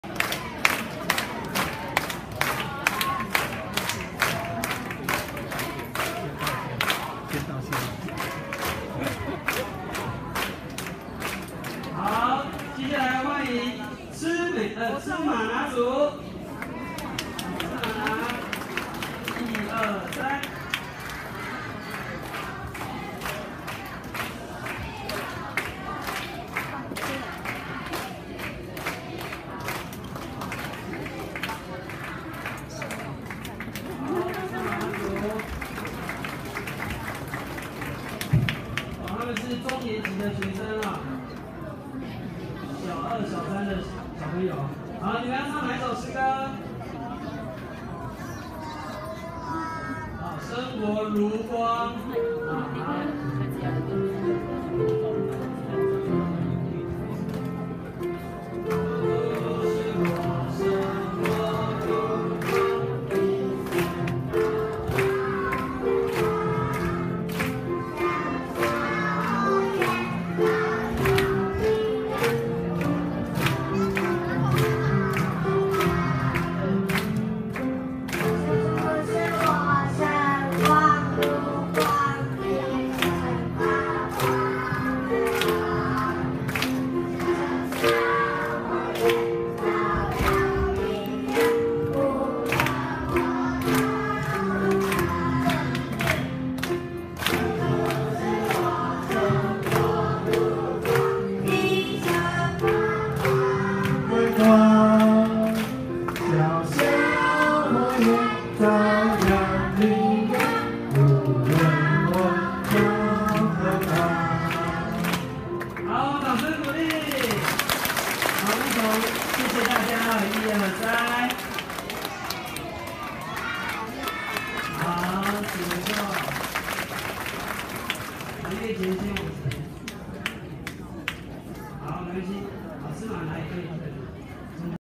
基隆市召會～兒童夏令營展覽～3《主題：摩西》2016/07/03
透過 YouTube 影片記錄《基隆市召會～兒童夏令營展覽》主題：摩西

時間：2016/07/03（日）09:00-11:30
地點：基隆市召會第一聚會所（基隆市信二路225號）
《2016兒童夏令營》
時間：7/1(五)-7/3(日)
對象：升幼稚園大班至升國五年級兒童